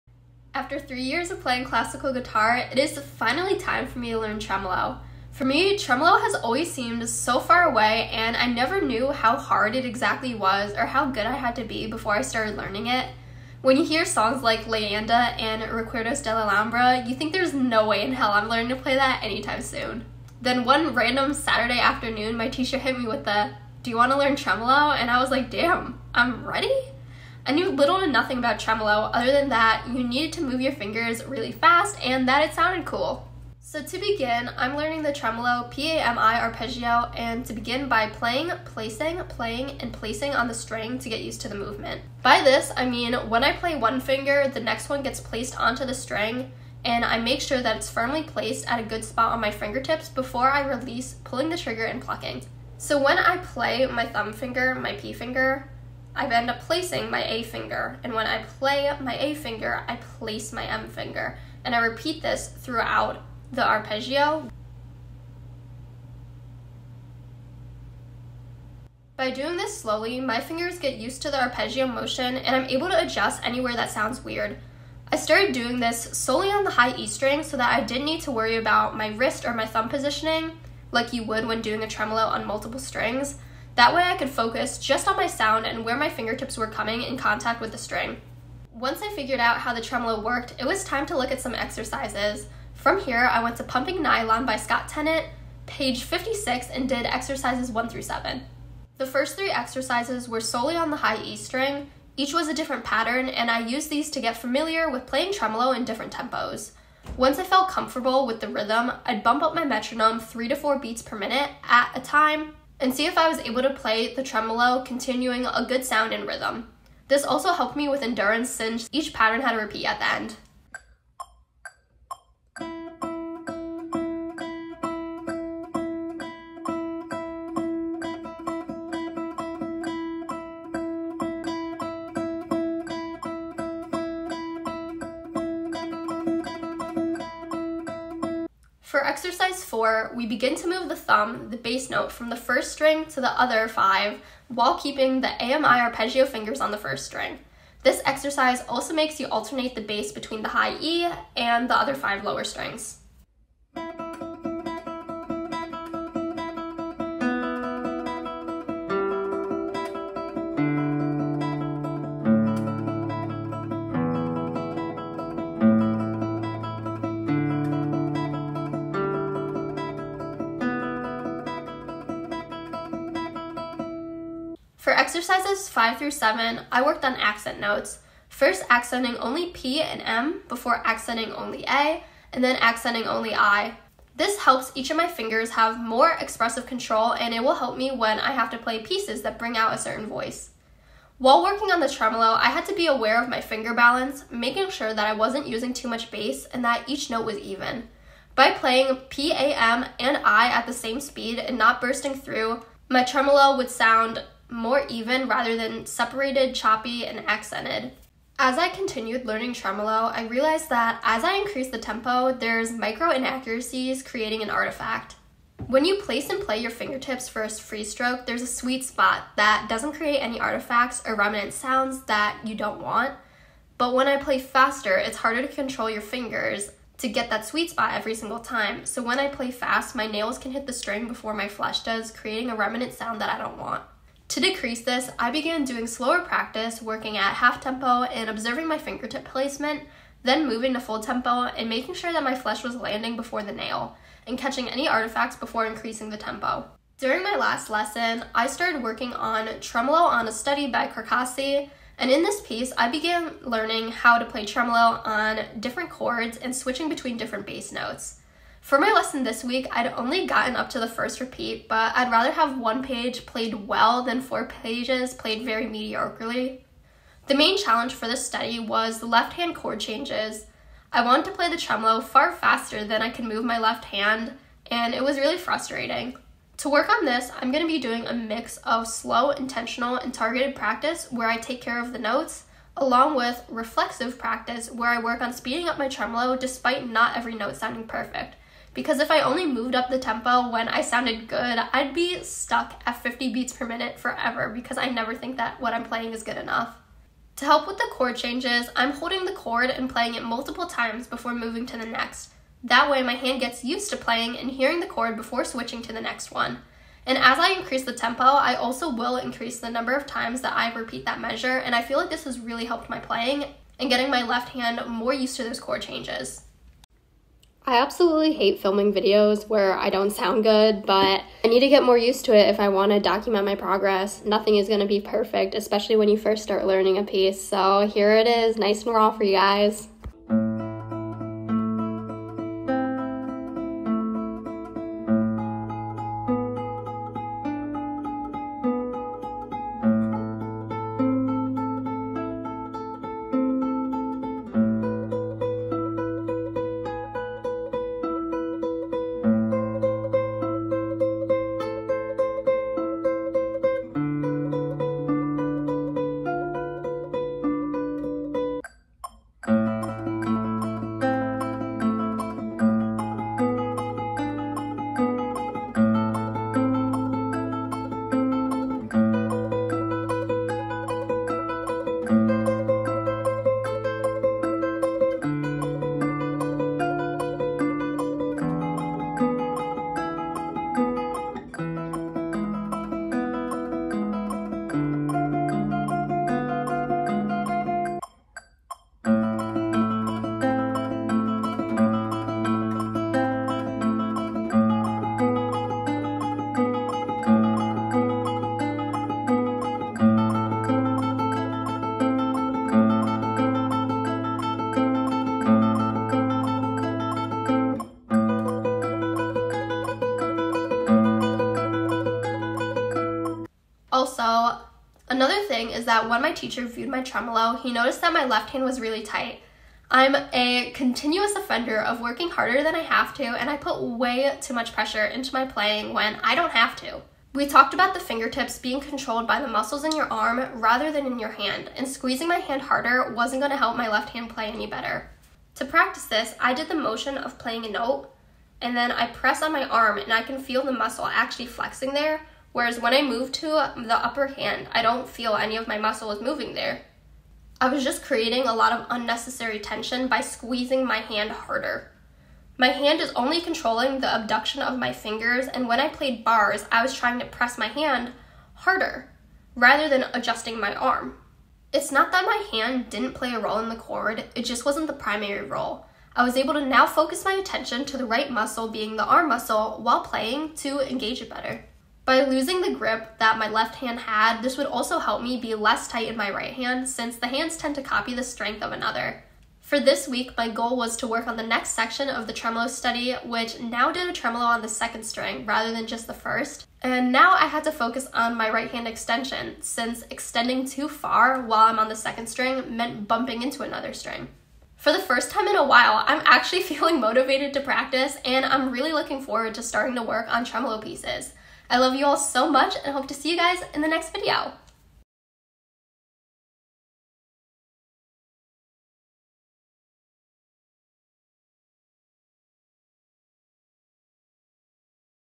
Learning Tremolo | CLASSICAL GUITAR VLOG #2
Learning and practicing tremolo for the first time!

Currently working on Pumping Nylon: The Classical Guitarist's Technique Handbook by Scott Tennant pg 56-58 exercises 1-7 as well as Tremolo on a Study by Matteo Carcassi.

One Year Classical Guitar Progress⤵